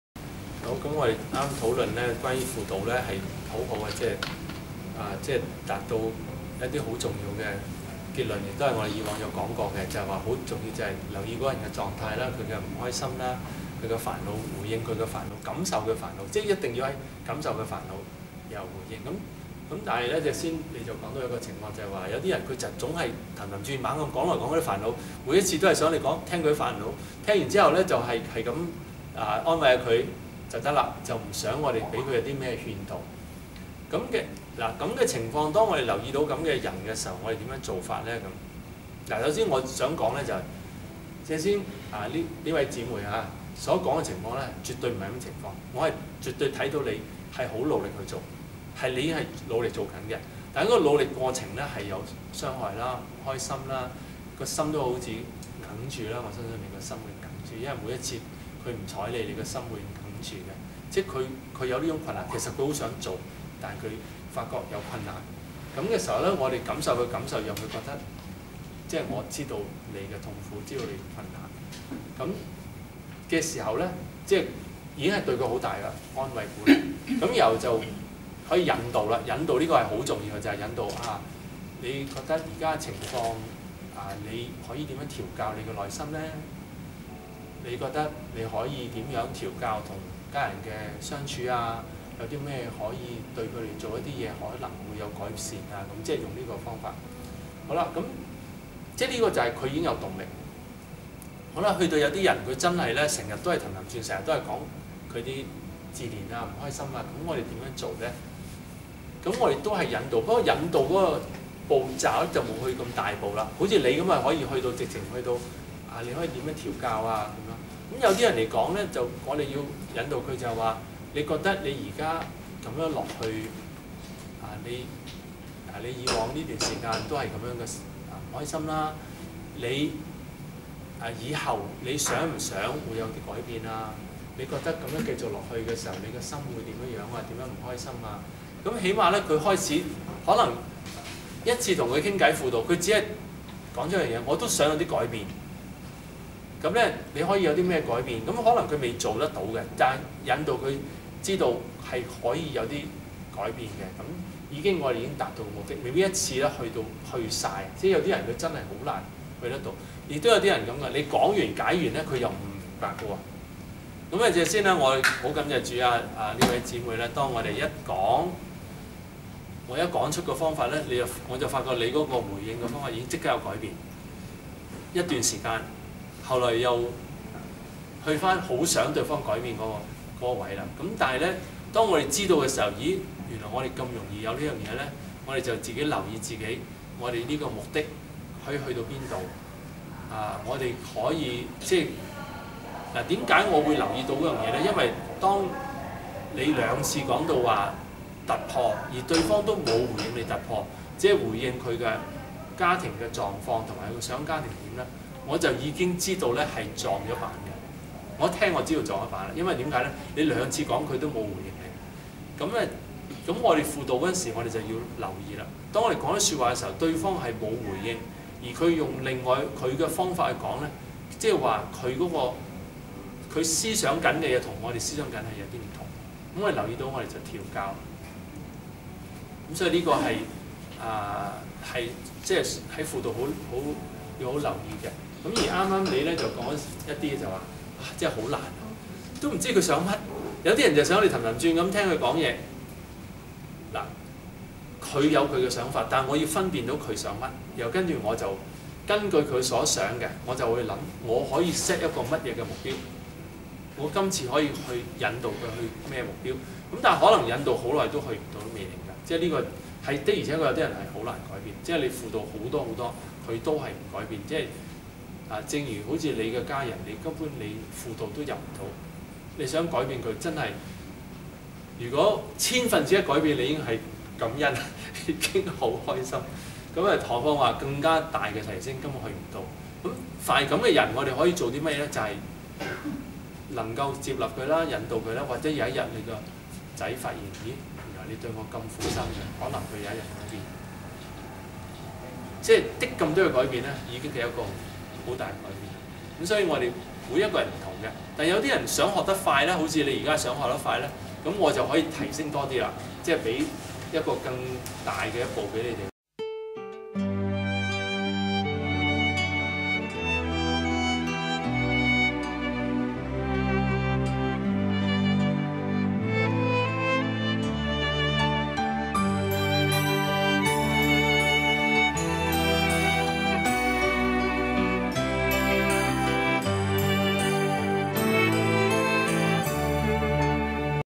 門徒訓練 –輔導一些常常重複自己困難的人的技巧 葉牧師
門徒訓練 –輔導一些常常重複自己困難的人的技巧
1．星期日：3:15崇拜；2:30服事禱告；崇拜後有小組及侍奉訓練課程。
2．星期一：4時服事禱告，7時侍奉訓練課程。